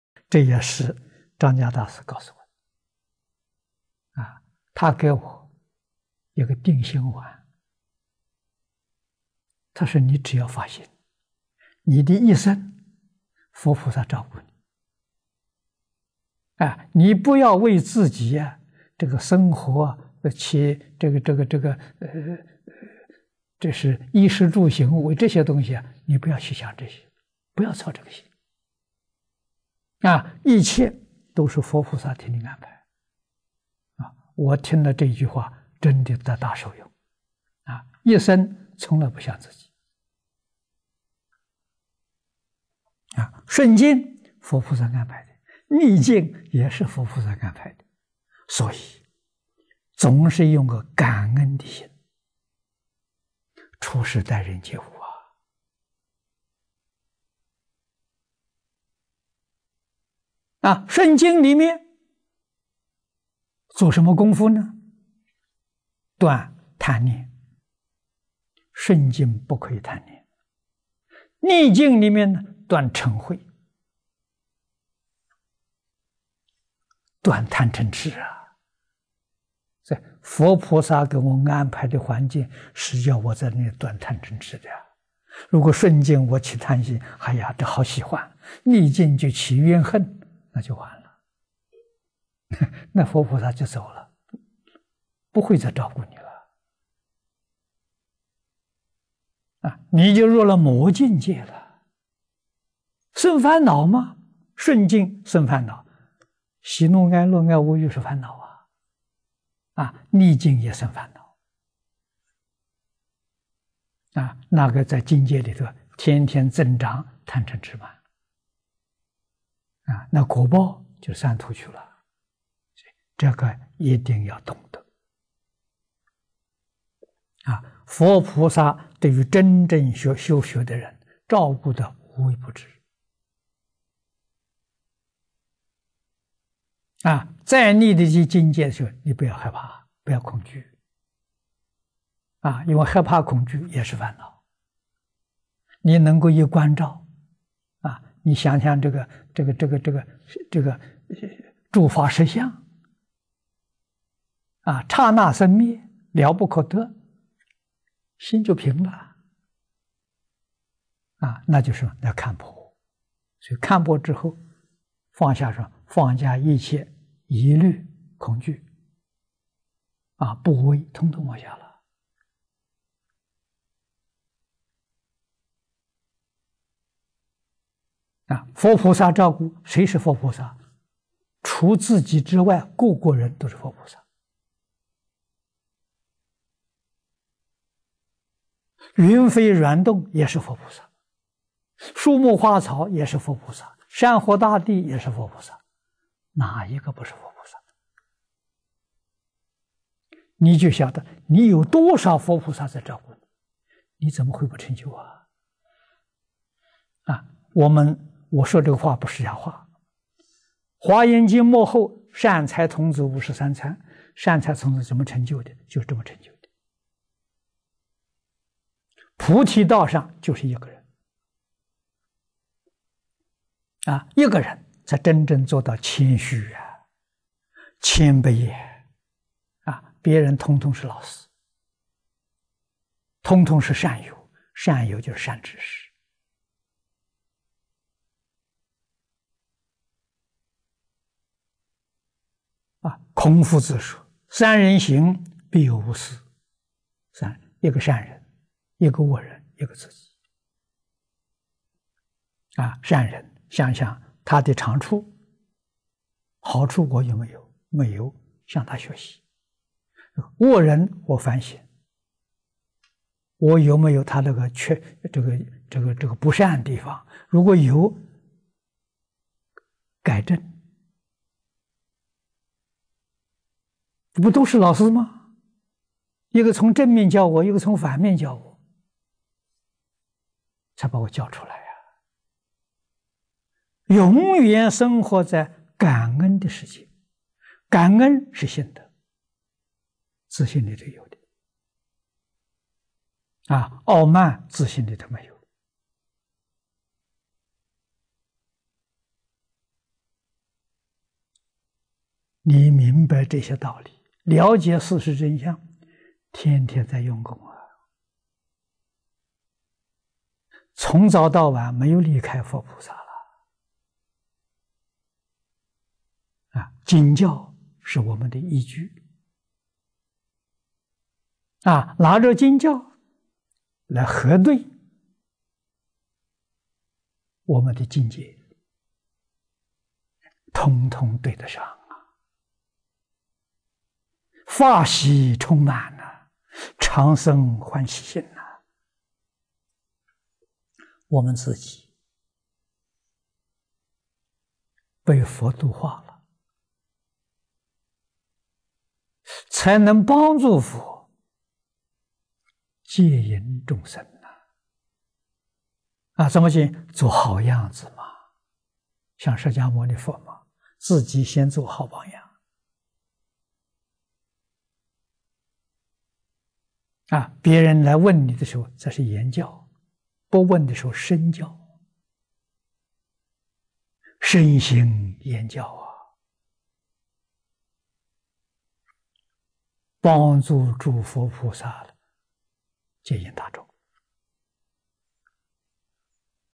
一生都交給佛菩薩安排
章嘉大師告訴我的，他給我一個定心丸，他說你只要發心，你的一生佛菩薩照顧你。你不要為自己的生活，衣食住行為這些東西，你不要去想這些，不要操這個心，一切都是佛菩薩替你安排。

我聽了這句話，真的得大受用，一生從來不想自己，順境是佛菩薩安排的，逆境也是佛菩薩安排的，所以，總是用個感恩的心處事待人接物。

順境裡面做什麼功夫？斷貪戀，順境不可以貪戀；逆境裡面斷瞋恚，斷貪瞋痴。所以佛菩薩給我們安排的環境，是要我在這裡斷貪瞋痴的。如果順境我起貪心，這好喜歡，逆境就起怨恨，那就完了，佛菩薩就走了，不會再照顧你，你就入了魔境界。生煩惱，順境生煩惱，喜怒哀樂愛惡欲是煩惱，逆境也生煩惱，那個在境界裡頭天天增長貪瞋痴慢，果報就三途去了。所以這個一定要懂得。

佛菩薩對於真正修學的人，照顧得無微不至，再逆的境界你不要害怕、不要恐懼，因為害怕、恐懼也是煩惱。你能夠一觀照，你想想，諸法實相剎那生滅，了不可得，心就平了，那就是什麼？要看破。所以看破之後，放下什麼？放下一切疑慮、恐懼、怖畏，統統放下。

佛菩薩照顧，誰是佛菩薩？除自己之外各個人都是佛菩薩，蜎飛蠕動也是佛菩薩，樹木花草也是佛菩薩，山河大地也是佛菩薩，哪一個不是佛菩薩。你就曉得，你有多少佛菩薩在照顧，你怎麼會不成就！

我說這個話不是假話，《華嚴經》末後善財童子五十三參，善財童子怎麼成就的？就這麼成就的。菩提道上就是一個人，一個人才真正做到謙虛、謙卑；別人統統是老師、統統是善友，善友就是善知識。孔夫子說「三人行必有我師」，三人，一個善人、一個惡人、一個自己。善人想想他的長處、好處，我有沒有？沒有，向他學習；惡人，我反省，我有沒有他那個不善地方？如果有，改正。不都是老師嗎？一個從正面教我，一個從反面教我，才把我教出來。

永遠生活在感恩的世界，感恩是性德，自性裡頭有的，傲慢自性裡頭沒有。你明白這些道理，了解事實真相，天天在用功，從早到晚沒有離開佛菩薩。經教是我們的依據，拿著經教來核對我們的境界，統統對得上，法喜充滿，常生歡喜心。

我們自己被佛度化，才能幫助佛接引眾生，怎麼接引？做好樣子。像釋迦牟尼佛自己先做好榜樣，別人來問你的時候才是言教，不問的時候身教，身行言教，幫助諸佛菩薩接引大眾。

無量壽經菁華 第一集